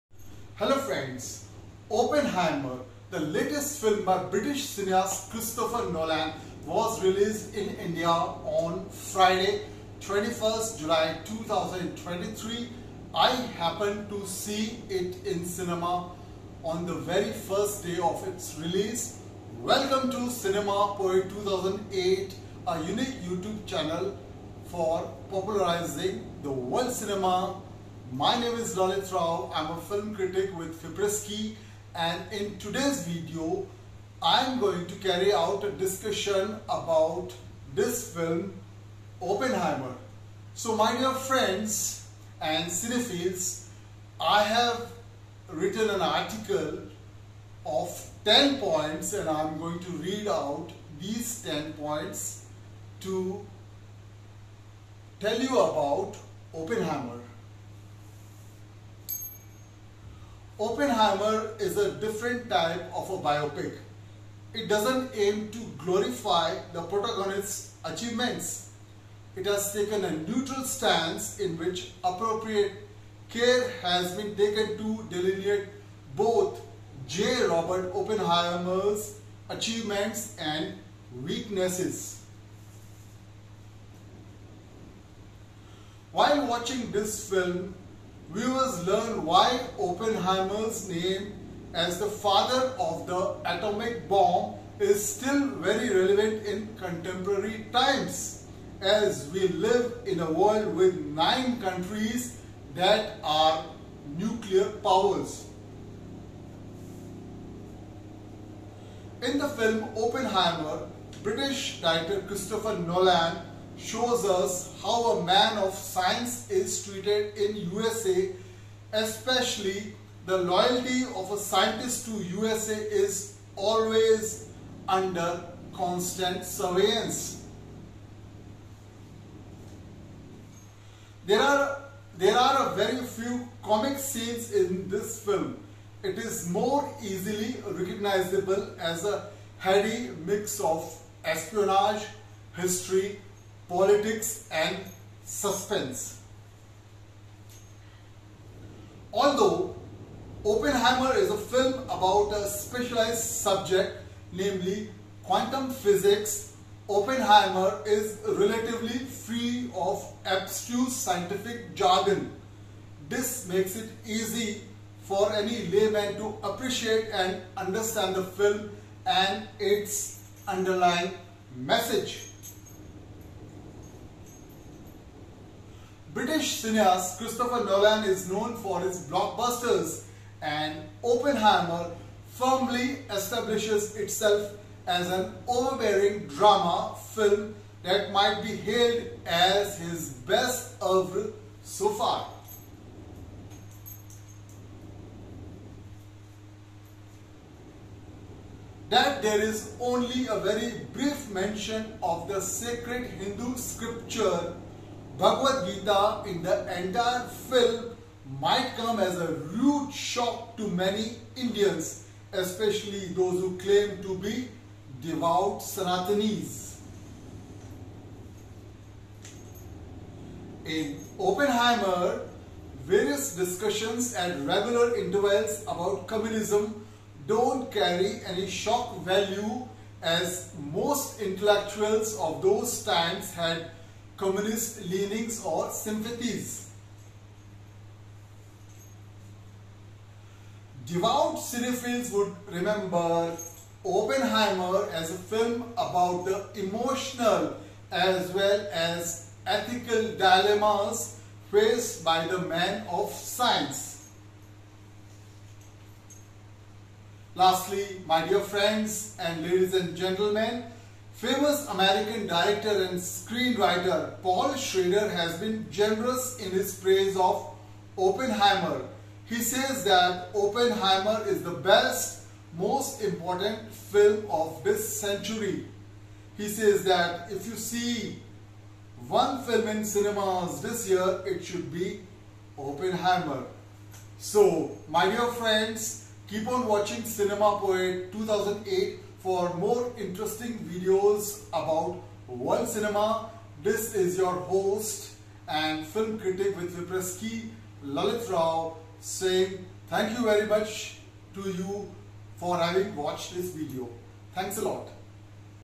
(23.07.2023) | Christopher Nolan's film OPPENHEIMER | reviewed by film critic Lalit Rao (FIPRESCI)
Oppenheimer (2023) directed by Christopher Nolan reviewed in 10 points.

1) Oppenheimer is a different type of a biopic. It doesn’t aim to glorify the protagonist’s achievements. It has taken a neutral stance in which appropriate care has been taken to delineate both J. Robert Oppenheimer’s achievements and weaknesses.

2) While watching this film, viewers learn why Oppenheimer’s name as the father of the atomic bomb is still very relevant in contemporary times as we live in a world with 9 countries that are nuclear powers!

3) In the film ‘‘Oppenheimer’’, British director Christopher Nolan shows us how a man of science is treated in USA especially the loyalty of a scientist to USA is always under constant surveillance.

4) There are a very few comic scenes in this film. It is more easily recognizable as a heady mix of espionage, history, politics and suspense.

5) Although it is a film about a specialized subject namely ‘‘Quantum Physics’’, Oppenheimer is relatively free of abstruse scientific jargon. This makes it easy for any layman to appreciate and understand the film and its underlying message.

6) British cinéaste Christopher Nolan is known for his blockbusters and Oppenheimer firmly establishes itself as an overbearing drama film that might be hailed as his best oeuvre so far.

7) That there is only a very brief mention of the sacred Hindu scripture Bhagavad Gita in the entire film might come as a rude shock to many Indians especially those who claim to be devout Sanatanis.

8) In ‘‘Oppenheimer’’, various discussions at regular intervals about Communism don’t carry any shock value as most intellectuals of those times had communist leanings or sympathies.

9) Devout cinéphiles would remember ‘‘Oppenheimer’’ as a film about the emotional as well as ethical dilemmas faced by the men of science.

10) Lastly, famous American director and screenwriter Paul Schrader has been generous in his praise of Oppenheimer. He says that Oppenheimer is the best, most important film of this century. He says that if you see one film in cinemas this year it should be Oppenheimer.